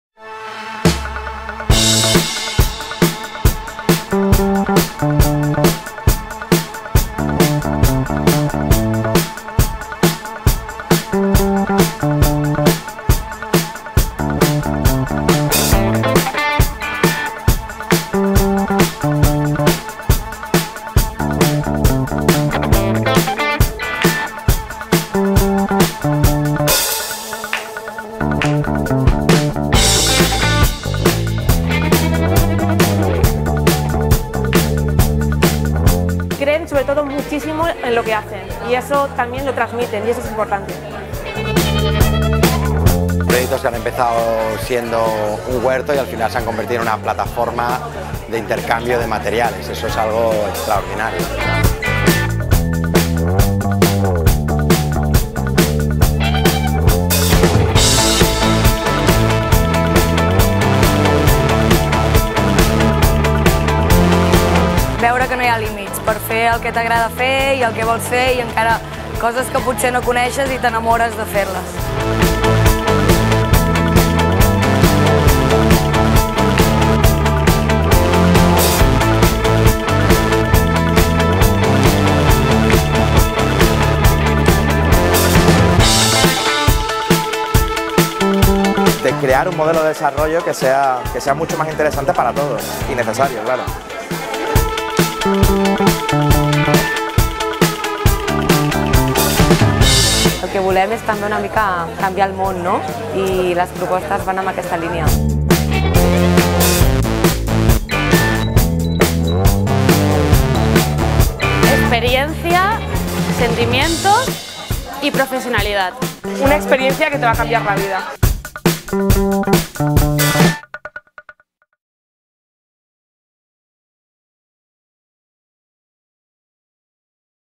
MID Talent: 20 jóvenes, un reto, inspiración, una semana de convivencia, formación...
¿Tienes menos de 30 años? ¿Quieres mejorar tu perfil profesional con formación específica en emprendimiento creativa impartida por profesionales de prestigio? ¿Te gustaría impulsar un proyecto y contar con el asesoramiento de un coach?

¡Súmate al programa MID Talent impulsado por BCD y vive una semana de inspiración y convivencia en un youth hostel de Barcelona con otros jóvenes con ganas de enfrontarse a retos y acelerar su carrera mediante la metodología design thinking y el trabajo en equipo!